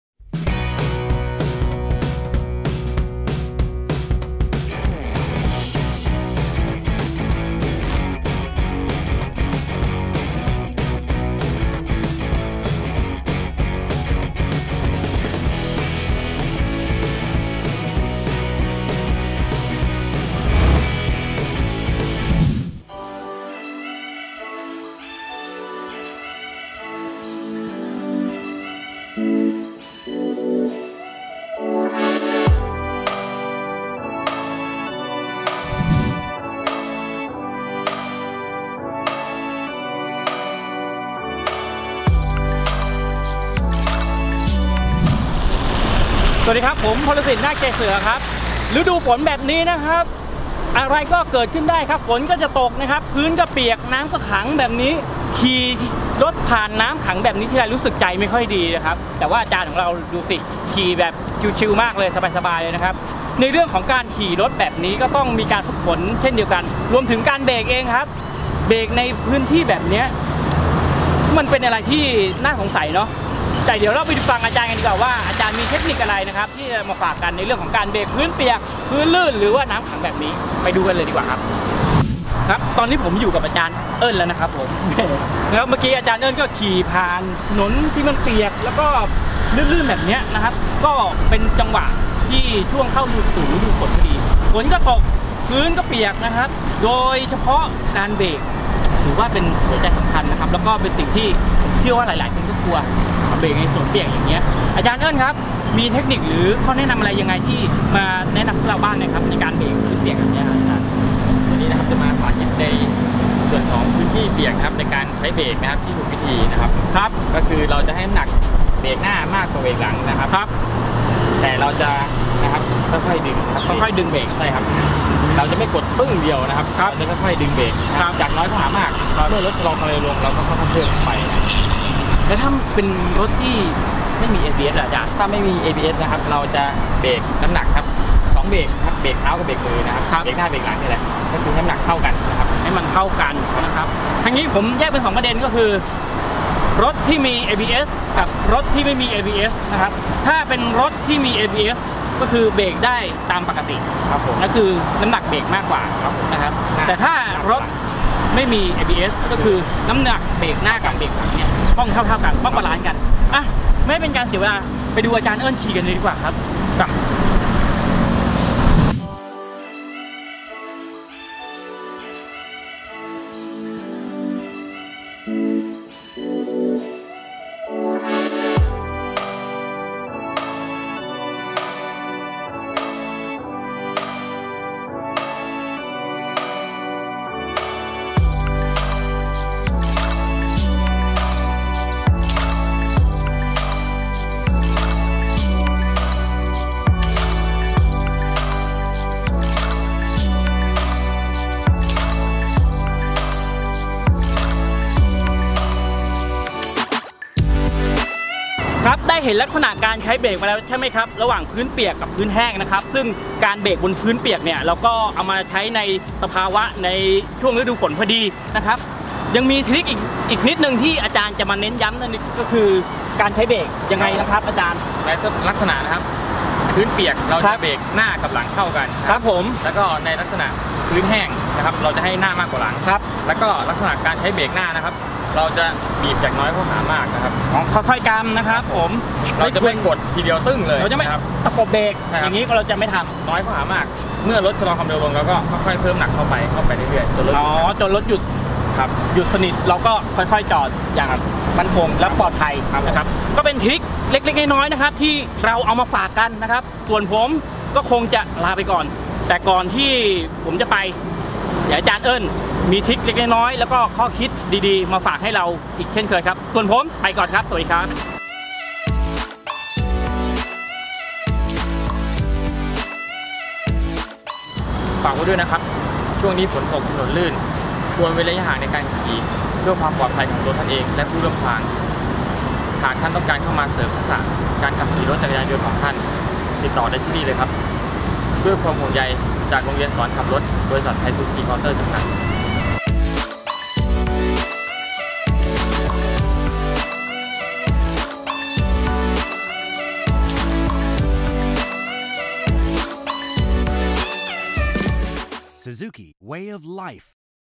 ฤดูฝนแล้ว...เบรกยังไงเมื่อถนนลื่น!!!
ทักษะในการขับขี่สำคัญเป็นอย่างยิ่ง และอีกหนึ่งสิ่งที่สำคัญเป็นอย่างมาก คือ การเบรกบนถนนที่เปียกลื่น ช่วงนี้เข้าสู่ฤดูฝนอย่างเต็มตัวแล้ว ซูซูกิ ขอแนะนำเทคนิคในการเบรกบนถนนเปียกลื่น มาให้ชมกัน

ด้วยความปารถนาดีจาก โรงเรียนสอนขับรถ บริษัท ไทยซูซูกิมอเตอร์ จำกัด
วันจันทร์ – วันศุกร์ เวลา 8.00 – 17.00 น.